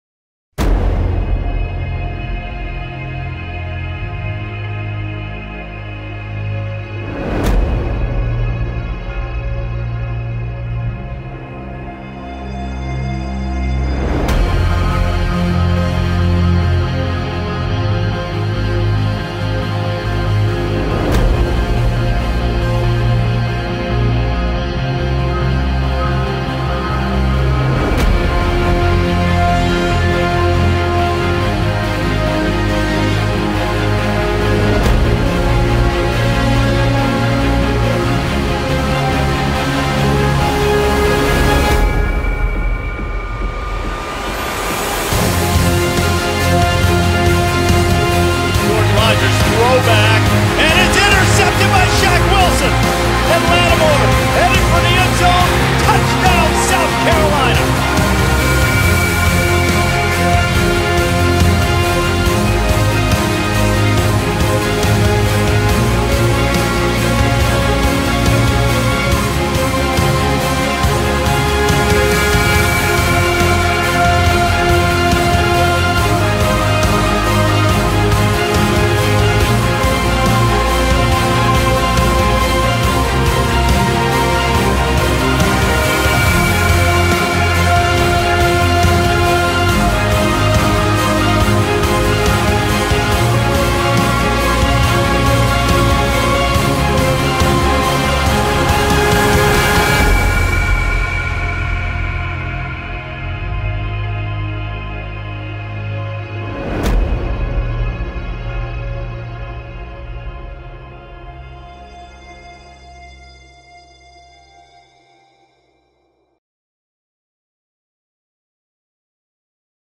South Carolina vs Vanderbilt 2012
South Carolina came out on top of it's opening match against Vanderbilt 17-13 thanks to a gutsy, gritty game by Connor Shaw and the strong return of Marcus Lattimore.